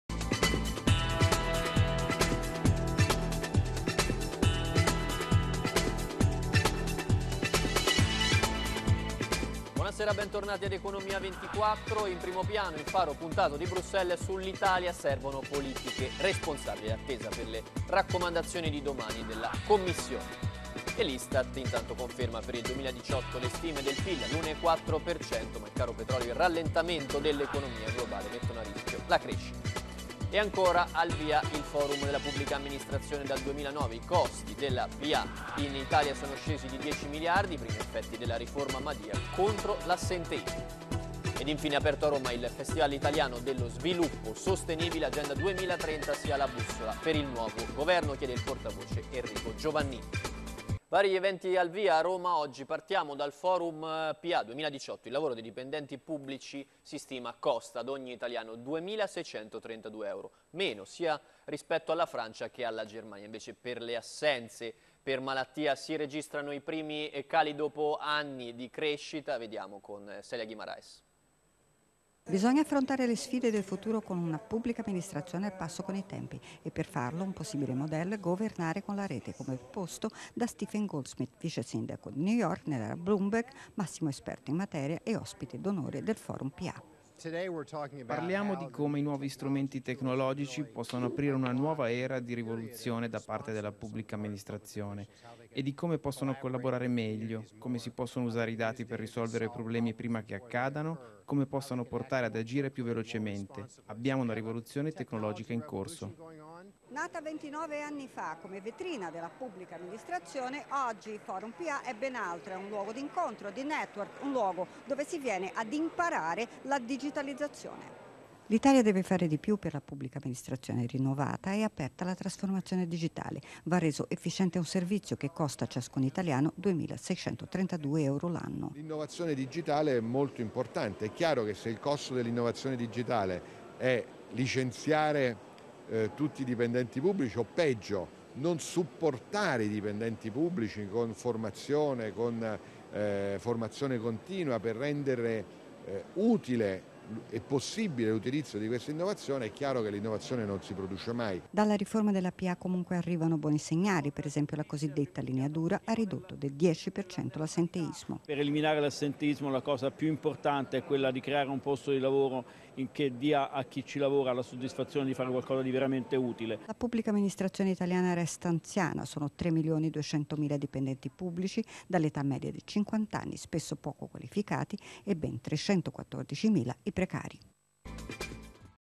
Rainews Economia: Cronache dal Forum PA 2018
Reportage dall'evento romano
di FPA dedicato all'innovazione digitale nella Pubblica Amministrazione. Stephen Goldsmith, docente alla Harvard University, sottolinea la possibilità di una nuova era per la PA grazie alle tecnologie. Nicola Zingaretti, governatore della Regione Lazio, parla della necessità di "comunicare" l'innovazione ai dipendenti pubblici. Carlo Mochi Sismondi, Presidente di FPA, commenta un'indagine che evidenzia il calo dell'assenteismo nella PA italiana.